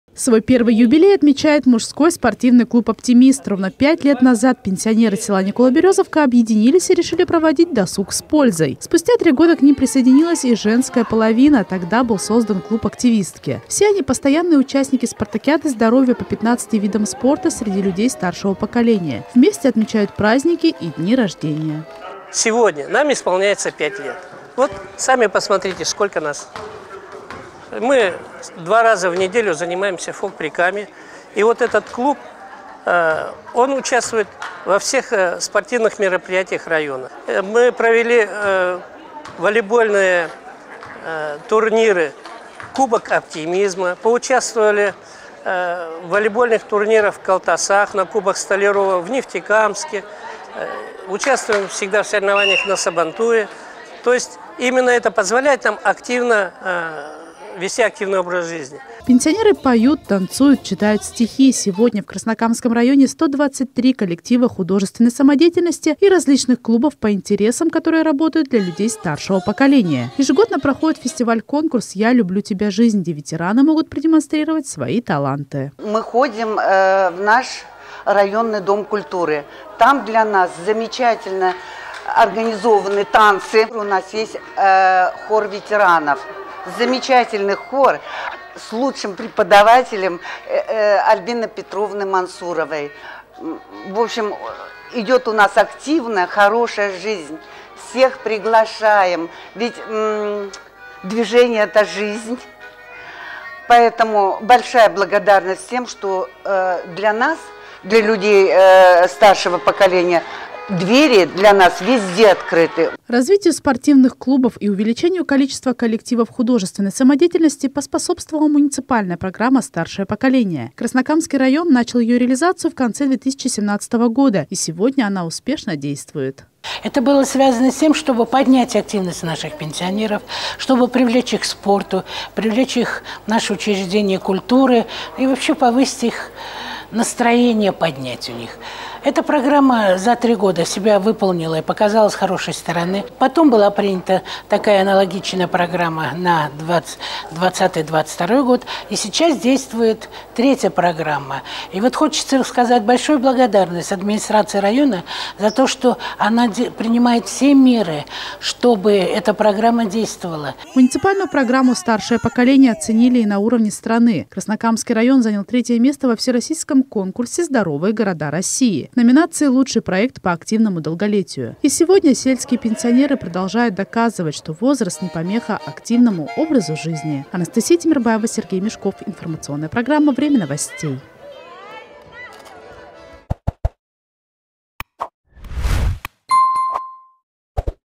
Активное долголетие
Краснокамский район занял 3 место во всероссийском конкурсе «Здоровые города России» в номинации «Лучший проект по активному долголетию». Напомним, в районе реализуется муниципальная программа «Старшее поколение», благодаря которой пенсионеры активно вовлечены в спортивную и социально-культурную жизнь. Подробности у Анастасии Тимербаевой.

Наши кнопки: Уфанет –  196 в «цифре» и 22 (!) кнопка в аналоге, Башинформсвязь – 403, Ростелеком – 761, Зеленая точка – 44